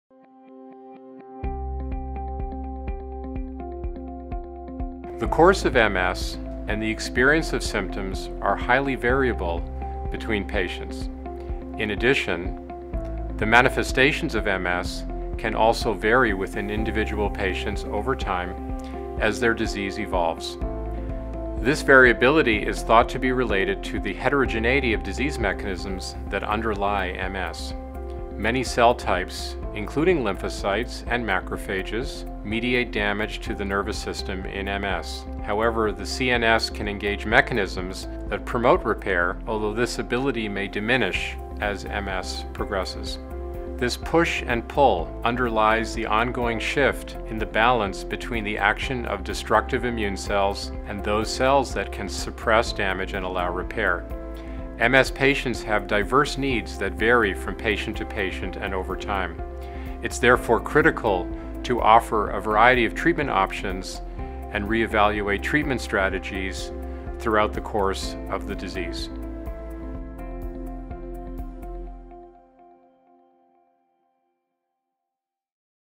Dr. Ralph Kern Discusses the Variability of Symptoms During the Course of MS
Dr. Ralph Kern, SVP-Head Worldwide Medical at Biogen, discusses the variability of symptoms during the course of Multiple Sclerosis (MS). In MS, there is an ongoing shift in the balance between the damaging immune cells and repair of the damage. Often, a variety of treatment options are needed throughout the changing course of the disease. Visit @BiogenMS and @Biogen on Twitter for more information and updates.